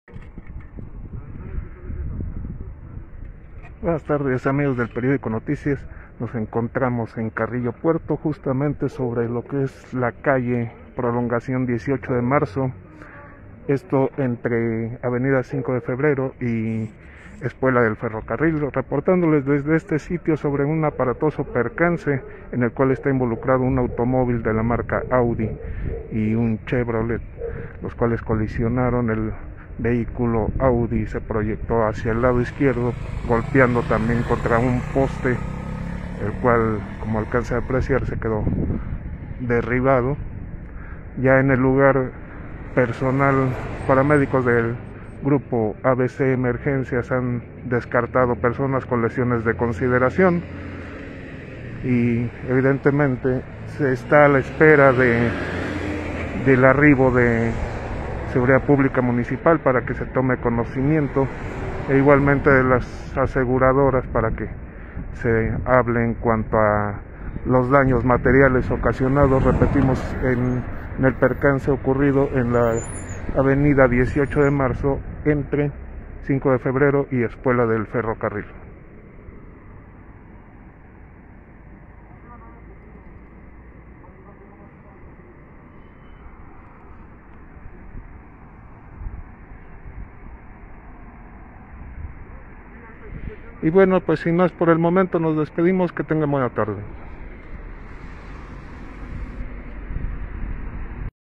Aparatoso choque en prolongación 18 de marzo
Aparatoso percance vehicular se registró ayer por la tarde en Carrillo Puerto, siendo dos los vehículos involucrados, por fortuna sin que se reportaran personas con lesiones graves.